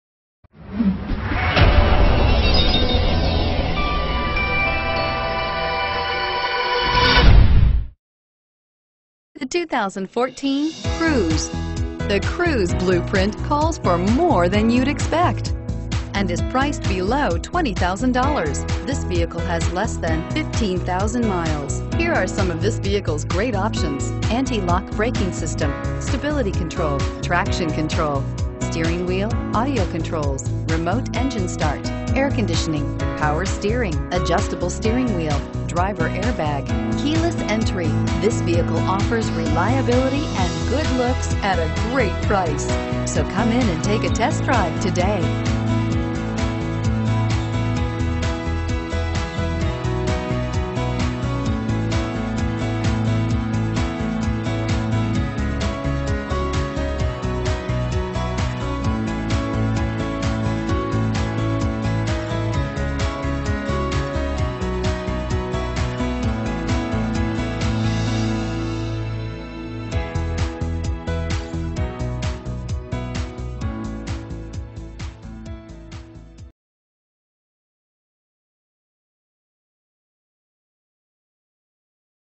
2014 Chevrolet Cruze Old Bridge Township NJ C7016R
2014 Chevrolet Cruze 2LT

Oasis Chevrolet
1292 U.S. 9

You'll love this 2014 Chevrolet Cruze. This is a 4dr Car you'll want to take home. With 13717 miles, it features Automatic transmission and an exterior color of Tungsten Metallic. Call and get in touch with Oasis Chevrolet directly, and be the first to open the 4dr Car door today!